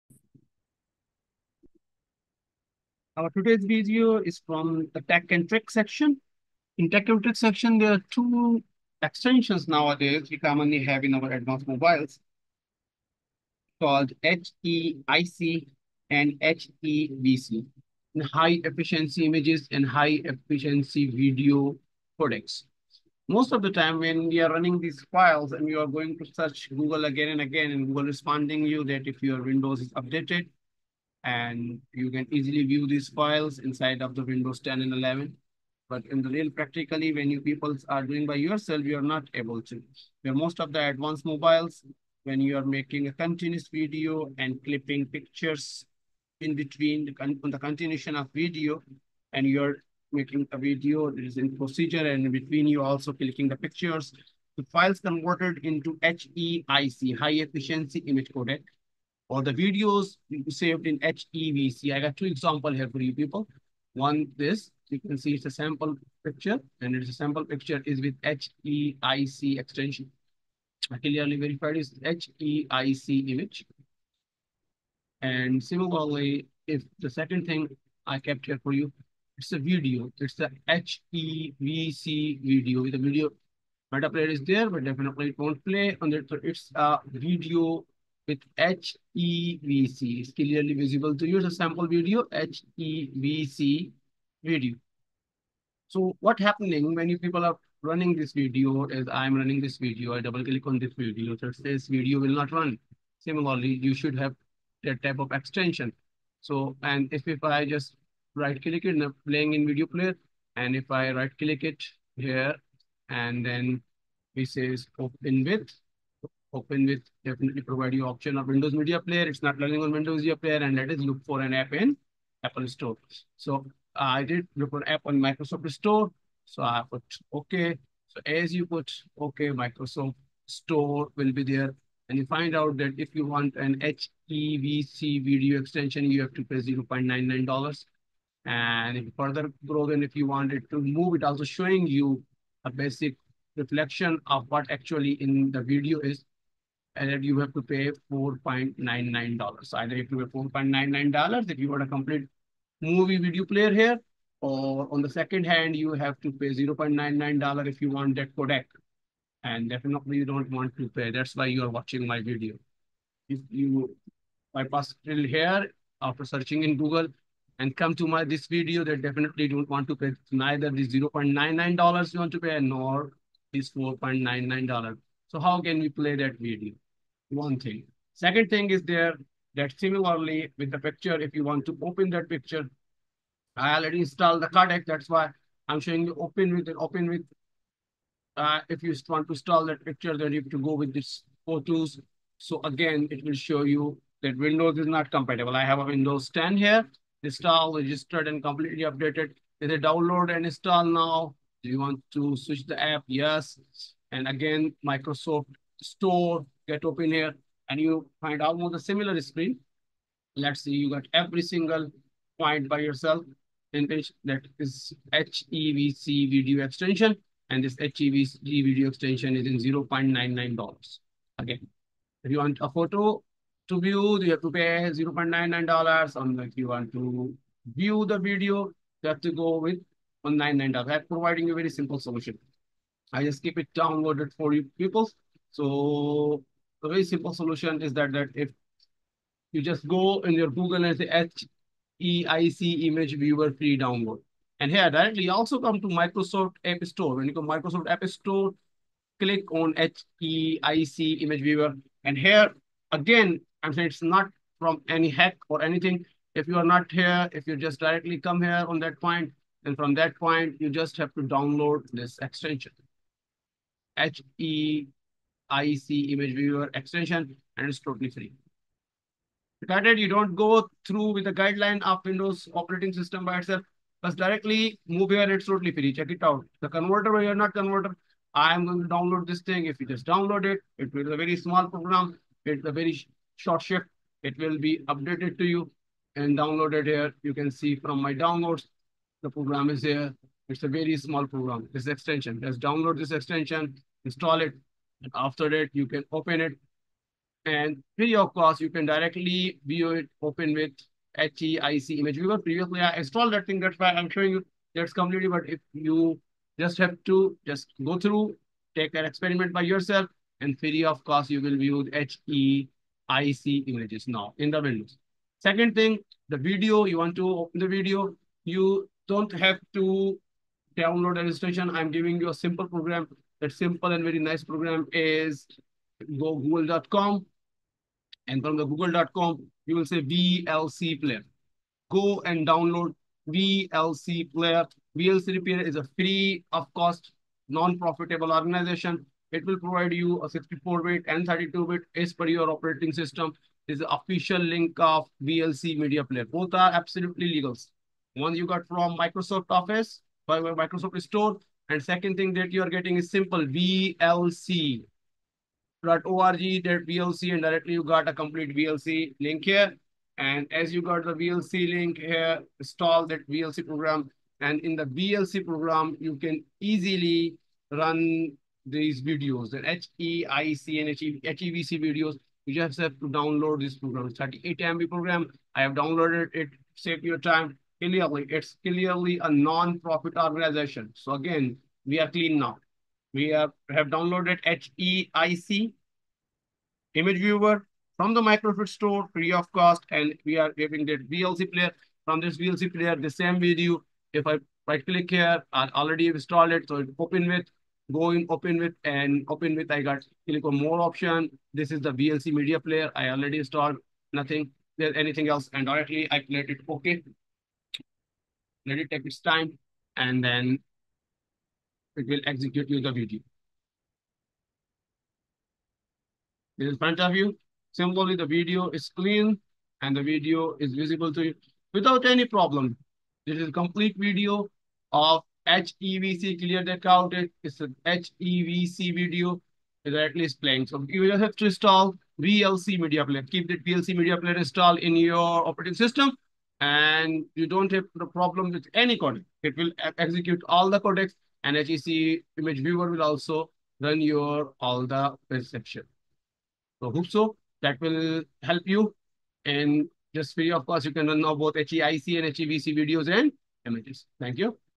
How to open HEIC & HEVC in Windows 10/11 for FREE legally.
Viewing HEIC - Images & HEVC Videos FREE in Windwos 10/11.